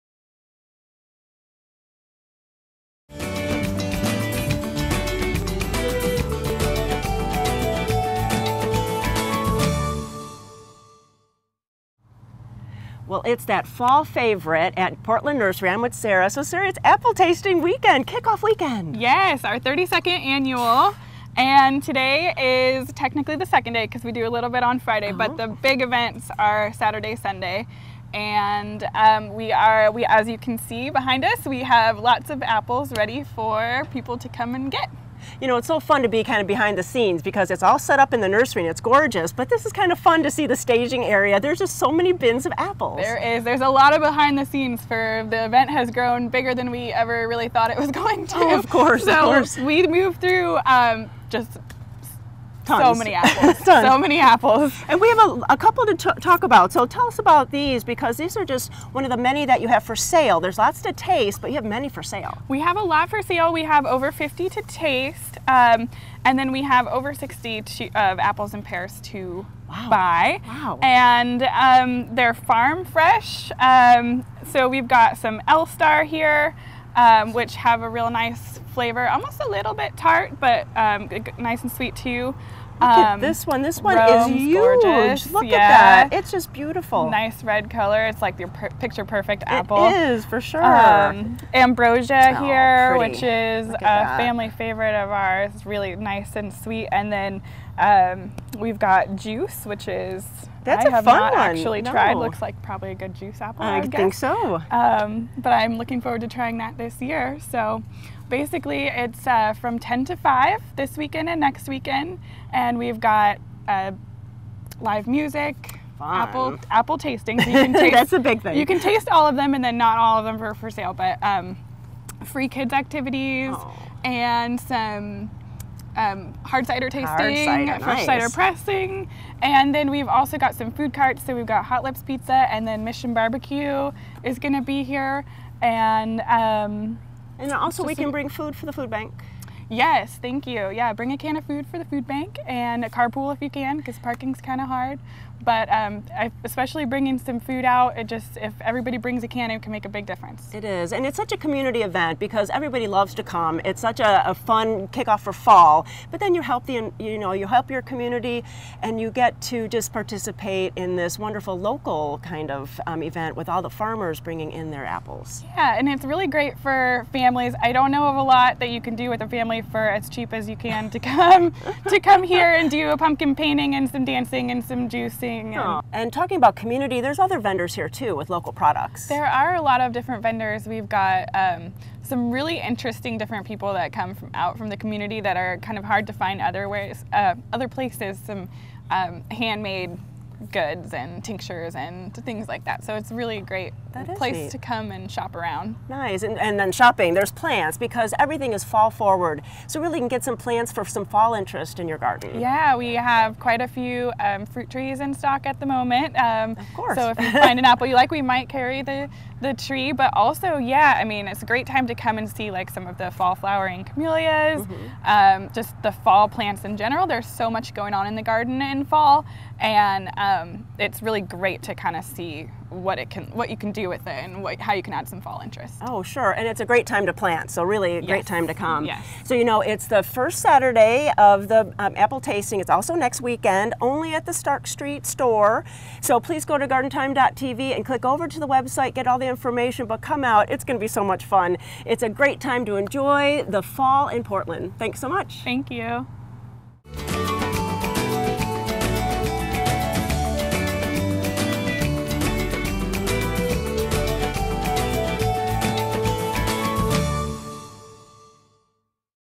Portland Nursery Apple Tasting
It is time for the 32nd annual Apple Tasting at Portland Nursery on Stark Street (in Portland) We visit with Sara to learn about what to expect!